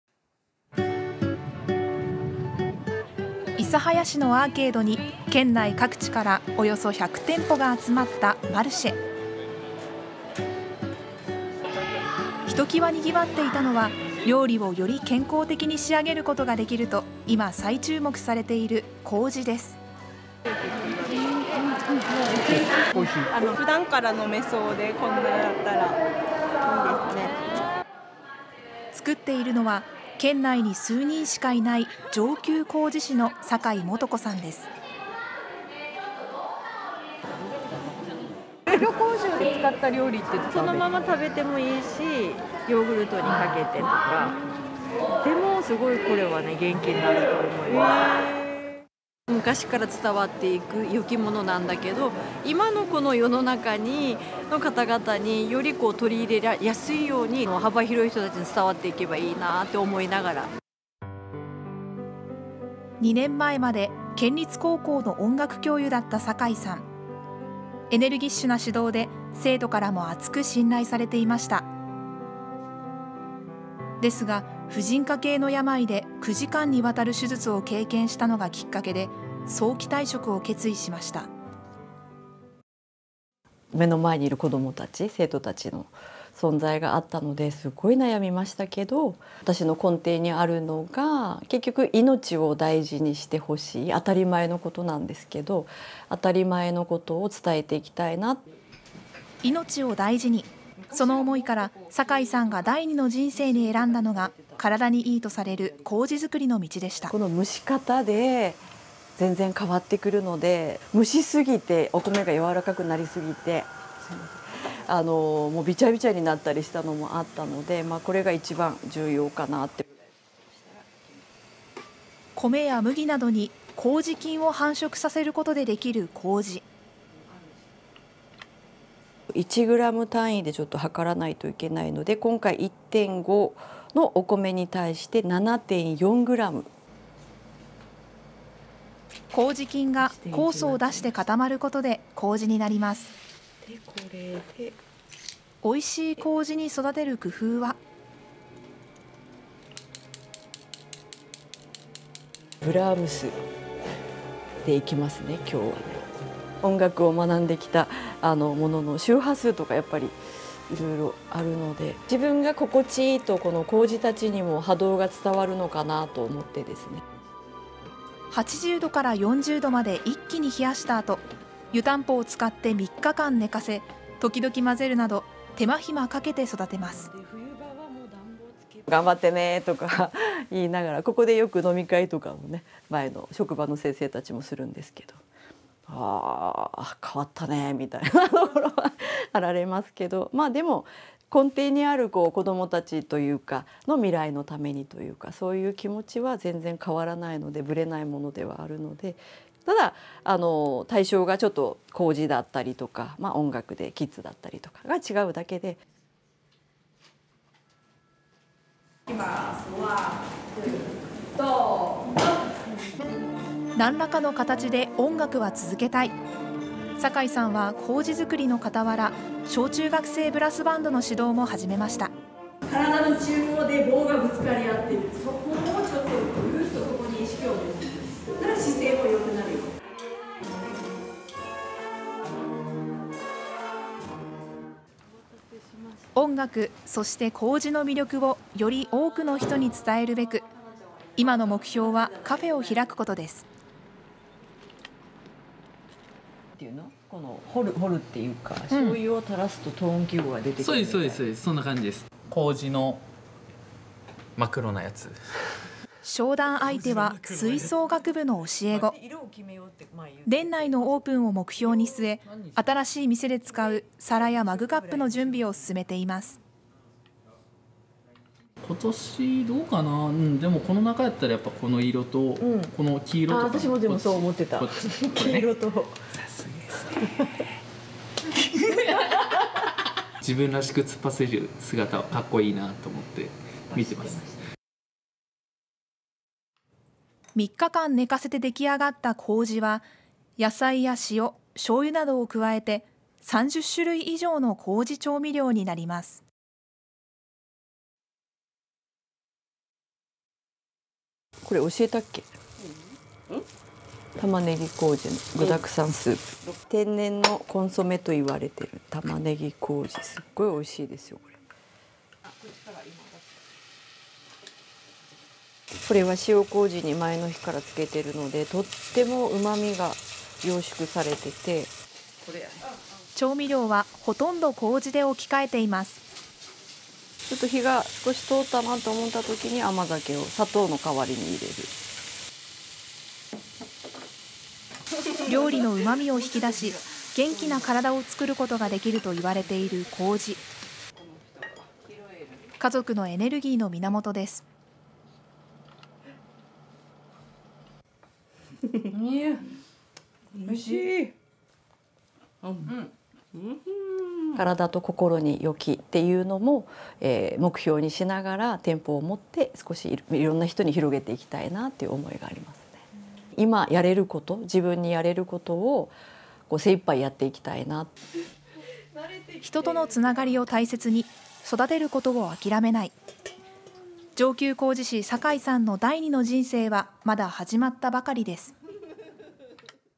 “体と心に良き” 麹づくり 上級麹士の元高校音楽教師の女性（54）が起業
約30年にわたって高校の音楽教諭をつとめた坂井泉子さんは2年前に早期退職し、麹づくりを手がける会社を長崎県大村市に立ち上げました。
坂井さんは長崎県内で数人しかいない“上級麹士”であり、その活動の根底には「子どもたちを育てる」情熱があります。
坂井さんは麹を用いた調味料や料理の製造に情熱を注ぎ、これまでに30種類以上の麹調味料を生み出しています。

昔から伝わる“良きもの”を取り入れやすく

長崎県諫早市のアーケードに県内各地からおよそ100店舗が集まったマルシェ。ひときわ賑わっていたのは、料理をより健康的に仕上げることができると いま再注目されている“麹”です。試食の客：
「おいしい」
「普段から飲めそうで こんなのだったら。いいですね」作っているのは県内に数人しかいない「上級麹士」の坂井泉子さん（54）です。客：「黒麹を使った料理っていうのは?」
坂井泉子さん：「そのまま食べても良いし、ヨーグルトにかけてとか。これは元気になると思います」
客：「へー」坂井さん：
「昔から伝わっている“良きもの”なんだけど、今のこの世の中に取り入れやすいように、より幅広い人に伝わっていけばいいなと思いながら」

音楽の“心地よさ”が麹にも伝わる

2年前まで県立高校の音楽教諭だった坂井さん、エネルギッシュな指導で生徒からも厚く信頼されていました。
しかし、婦人科系の病で9時間にわたる手術を経験したのがきっかけで、早期退職を決意しました。坂井さん：
「目の前にいる子どもたちの存在があったのですごく悩みましたけど。私の根底にあるのが”命を大事にしてほしい”当たり前のことなんですけど、当たり前のことを伝えていきたいなって」『命を大事に』その思いから坂井さんが第二の人生に選んだのが体に良いとされる麹づくりの道でした。坂井さん：
「“蒸し方”で全然変わってくるので、蒸しすぎてお米が柔らかくなりすぎて、べちゃべちゃになったりしたのもあったので、これが一番重要かなと」米や麦などに麹菌を繁殖させることでできる“麹”。坂井さん：
「1グラム単位で量らないといけないので、今回1.5キロのお米に対して7.4グラム」“麹菌”が酵素を出し、固まることで“麹”になります。
美味しい麹に育てる工夫は…（かく拌作業中、室内にクラシック音楽が流れる）坂井さん：
「ブラームスで行きますね、今日は。音楽を学んできたものの周波数とかあるので。自分が心地いいと麹たちにも波動が伝わるのかなと思ってですね」80度から40度まで一気に冷やした後、湯たんぽを使って3日間寝かせ、時々混ぜるなど手間暇かけて育てます。坂井さん：
「”頑張ってね”とか言いながら。ここでよく飲み会とか、前の職場の先生たちとするんですけど ”あ～変わったね”と驚かれますけど。
でも根底にある“子どもたちの未来のために（育てる）”という気持ちは全然変わらないのでブレないものではあるので、ただ対象が麹だったりとか音楽で違うだけで」

音楽指導も続けながらカフェの準備

『何らかの形で音楽は教え続けたい』坂井さんは麹づくりのかたわら、小中学生ブラスバンドの指導もはじめました。坂井さん：
「体の中央で棒がぶつかり合っている。そこにぐーっと意識を持って行く。そうしたら姿勢が良くなるよ」音楽、そして麹の魅力をより多くの人に伝えるべく、いまの目標はカフェを開くことです。（店内で使用する器の打合せ）坂井さん：
「醤油をたらすとト音記号が出てくる（皿）みたいな？」
浜陶　営業 太田海斗さん：
「そうです、そうです。そんな感じです」商談相手は、波佐見高校に赴任していた頃の吹奏楽部の教え子です。
年内のオープンを見据え、新しい店で使う皿やマグカップの準備を進めています。太田さん：
「今年の流行はどうかな？でもこの中だったらこの色（水色・紺）と黄色とか」
坂井先生：「私もそう思ってた！黄色と」
太田さん：「さすがですね～（タッチ）」太田さん：「自分らしく突っ走る姿はかっこいいなと思って見ています」

調味料はほとんどが麹　家族のエネルギーの源

3日間寝かせてできあがった麹は、野菜や塩、醤油などを加えて30種類以上の“麹調味料”になります。（台所で娘・桃子さん（27）と一緒に調理作業）坂井さん：
「これ教えたっけ?玉ねぎ麹の具沢山スープ。天然のコンソメといわれている『玉ねぎ麹』すごい美味しいですよ、これ」坂井さん：
「これ（肉）は塩麹に前の日からつけているので、とっても旨味が凝縮されていて」調味料はほとんど麹で置き換えています。坂井さん：
「火が少し通ったなと思った時に甘酒を砂糖の代わりに入れる」料理のうまみを引き出し、元気な体をつくることができるといわれている麹──家族のエネルギーの源です。（孫の新くんに料理を食べさせる娘・桃子さん）桃子さん：「おいしー」坂井さん：
「“体と心に良き”っていうのを目標にしながら、店舗をもって色々な人に広げていきたいなって思いがあります。今やれること自分にやれることを精いっぱいやっていきたいなと」人とのつながりを大切に”育てる”ことを諦めない。
上級麹士、坂井さんの第二の人生はまだ始まったばかりです。